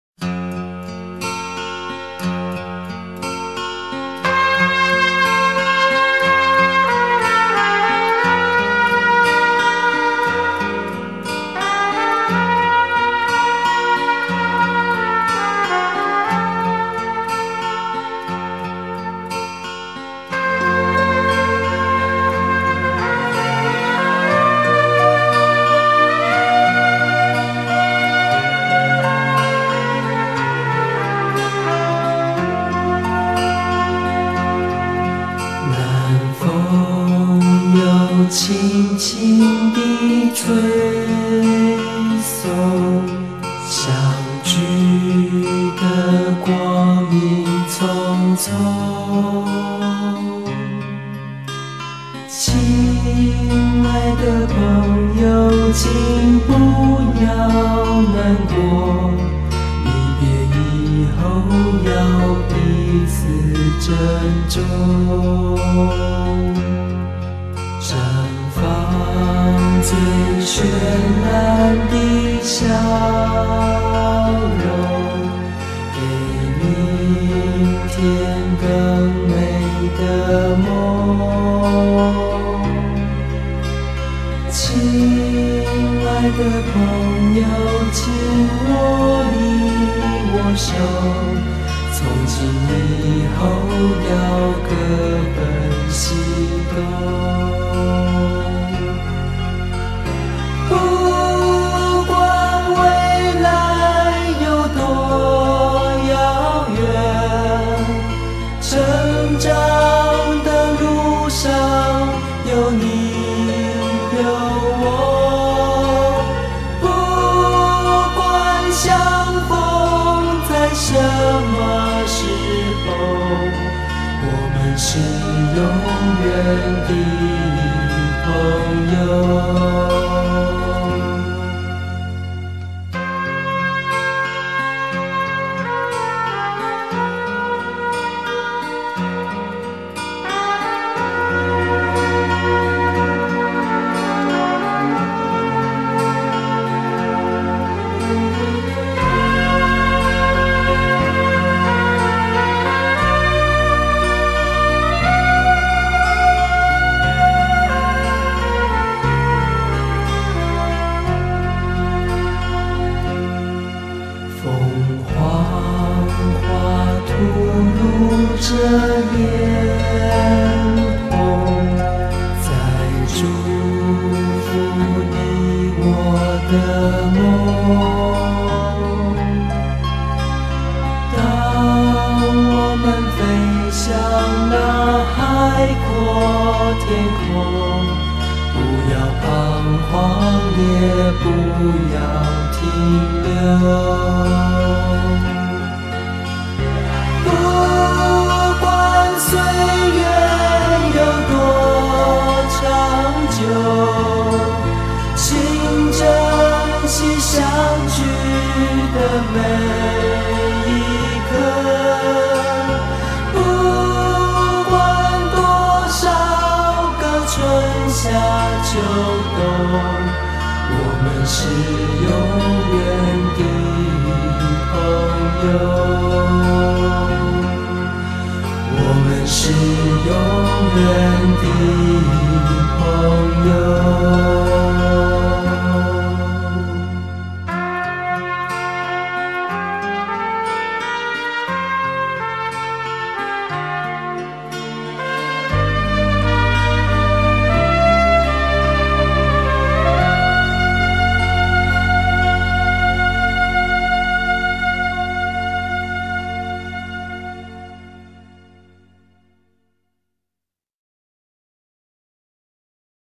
麻坡中华基督学校-- Graduates
毕业生校园内与家人合影。